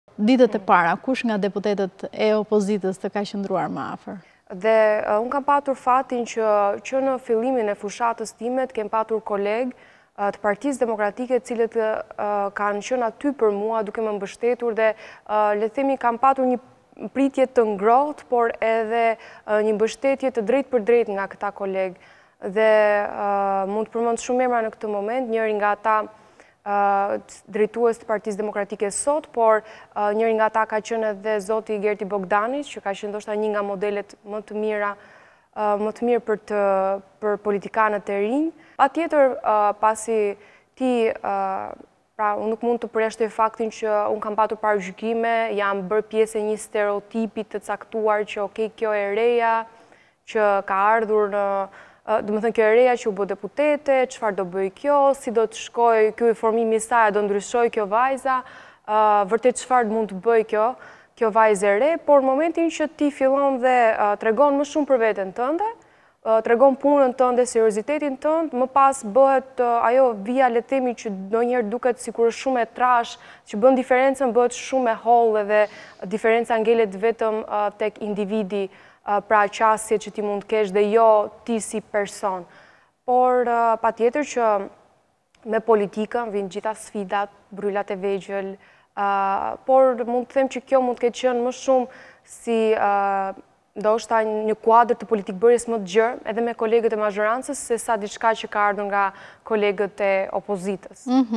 Deputetja Andia Ulliri flet për marrëdhëniet me kolegët dhe bërrylat
E ftuar ditën e sotme, në studion e lajmeve në "Ora News", ishte deputetja e PD, Andia Ulliri, për të folur për sfidat e të rinjve në politikë.

"Të rinjtë në politikë janë një grup që duhet të luftojë pak më shumë për zërin e tyre. Partitë politike në Shqipëri kanë strukturat e tyre që bëjnë të mundur që zëri i të rinjve të dëgjohet dhe ata të kenë zërin e tyre, jo vetëm në strukturat e partisë por ndonjëherë edhe si përfaqësues të qytetarëve siç është edhe të qenurit deputet në rastin tim. Mendoj që të rinjtë dhe politika janë diçka dinamike, sepse të rinjtë janë të zjarrtë për vizionin që ata kanë për vendin e tyre, për idealet që ata gëzojnë. Jo gjithmonë politika që ne shohim në Shqipëri e bën të mundur që ti këto ideale që ke ti vësh në zbatim, por e rëndësishme është që të rinjtë duhet të jenë ata që duhet ta bëjnë faktike që dëshira e tyre është të jenë aty, të japin zërin e modelin e tyre e të përcjellin mesazhin që sapo të rinjtë të jenë pjesë e politikës, politika do duhet të jetë e sforcuar që ti dsëgjojë këta të rinj e ti marr në konsideratë. Besoj se ka akoma nevojë të bëhet pak më shumë punë.